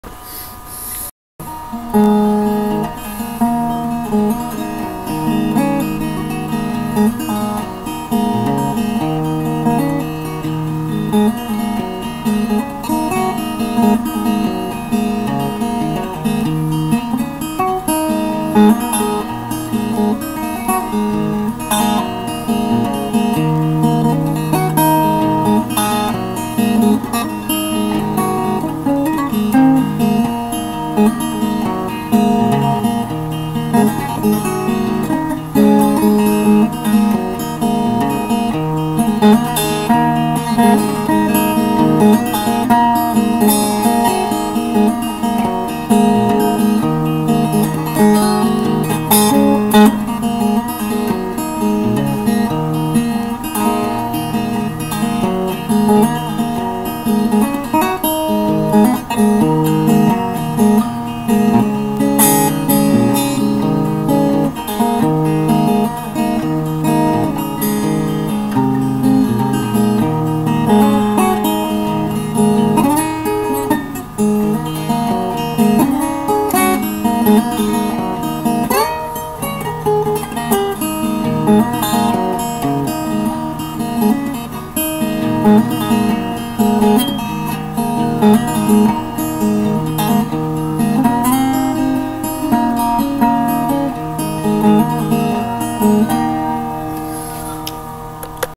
Bit of fun old timer
messing with cheese on linux to capture me playing guitar , like the outcome actually...